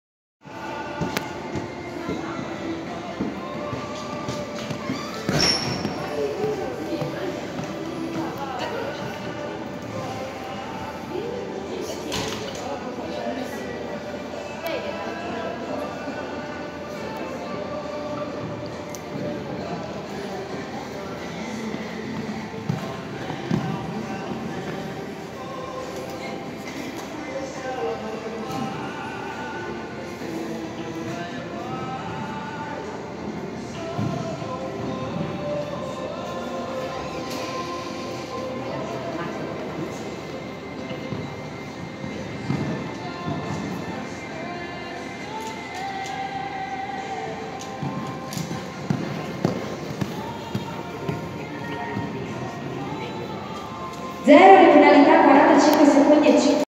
Io e Grupo Promo Alandia S
Io e Grupo Promo Alandia S, Holsteiner del 2008 (Cormint x Corrado II), Naz.A5* Vermezzo 3 febbraio 2018, Piccolo GP (h135 mista)